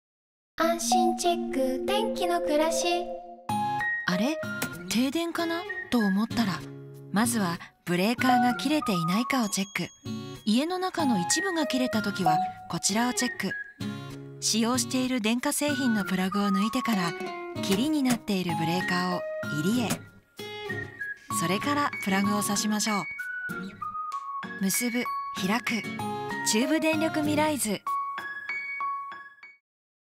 【中部電力ミライズ】ブレーカー篇
停電時のブレーカー操作をご案内しています。